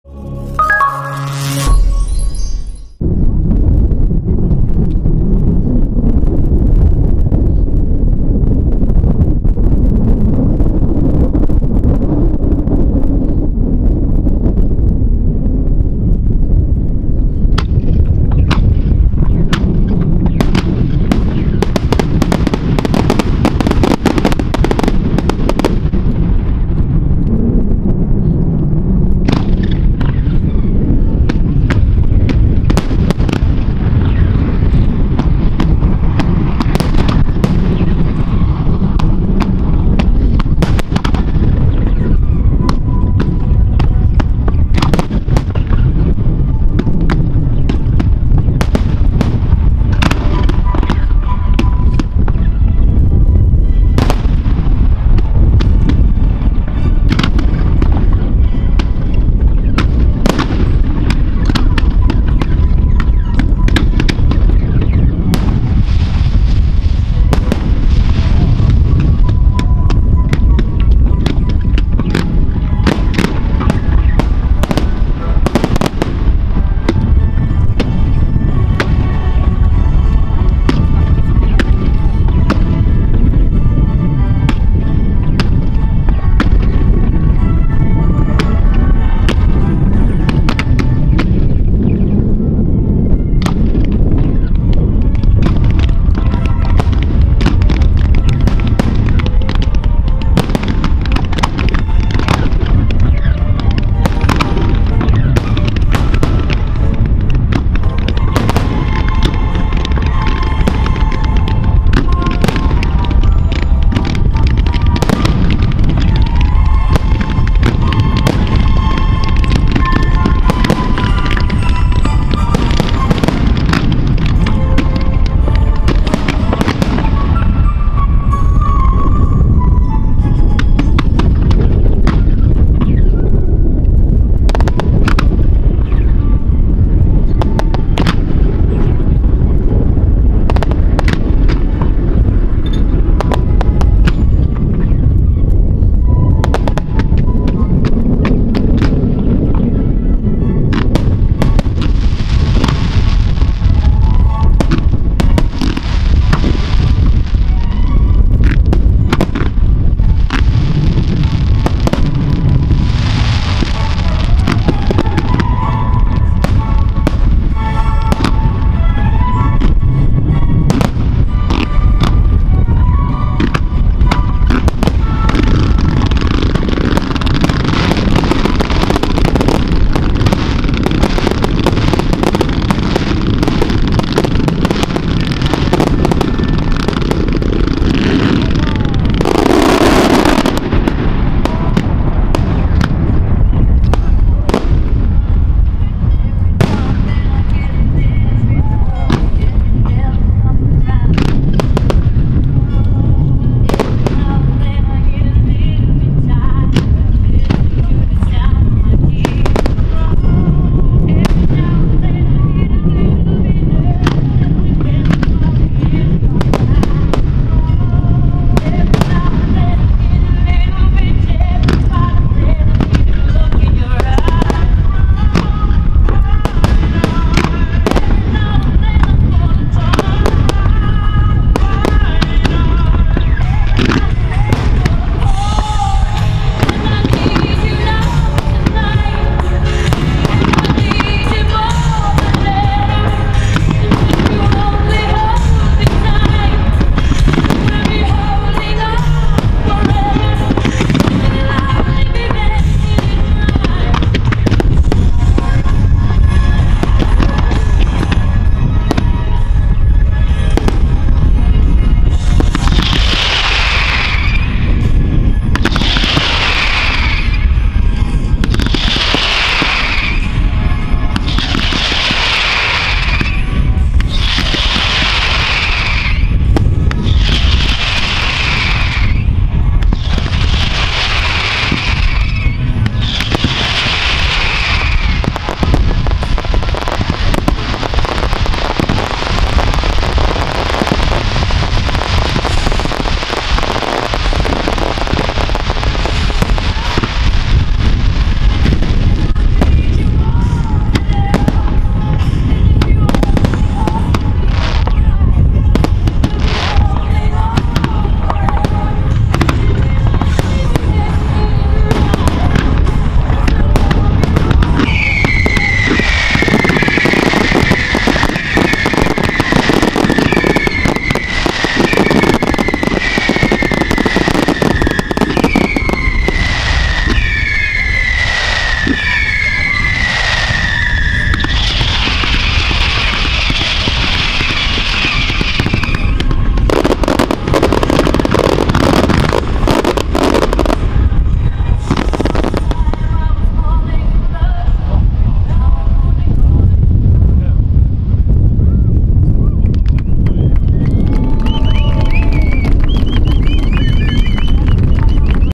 Jorge Feuerwerk Abschlußshow / Pyroforum 2015
Sehr schöne Show, passend zur Musik, definitiv ein klasse Feuerwerk  / Produktpräsentation

Leider gab es an diesem Abend sehr starken Wind und Regen. Es ist leider unübersehbar =)